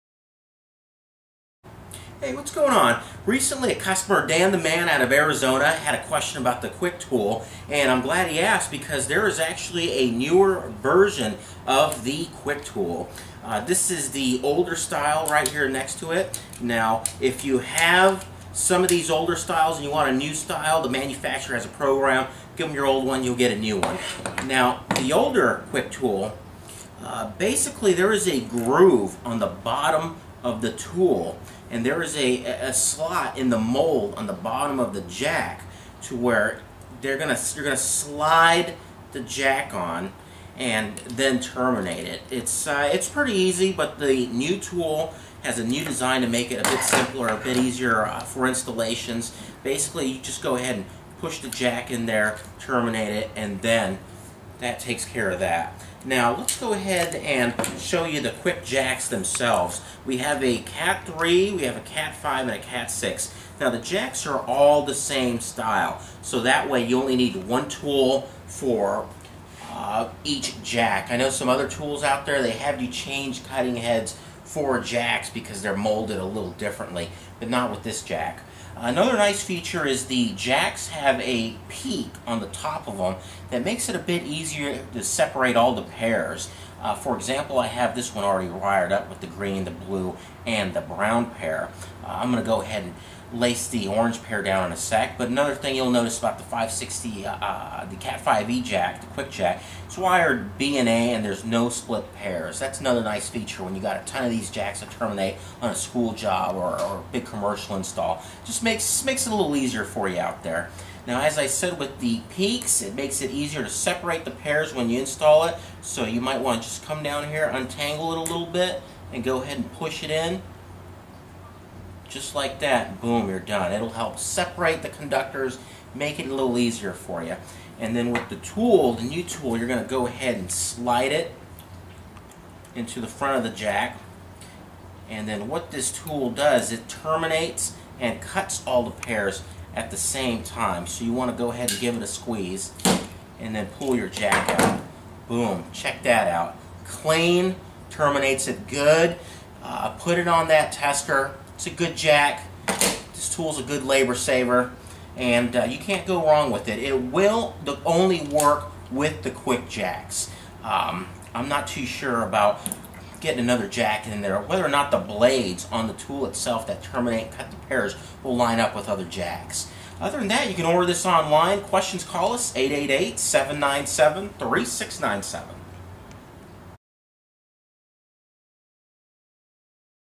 How to Punch Down a Cat5e Cat6 Kwik Jack
Order online.The new kwik tool makes for terminating Ethernet insert jacks easier. The tool will cut and terminate all the pairs at the same time. Save labor and time!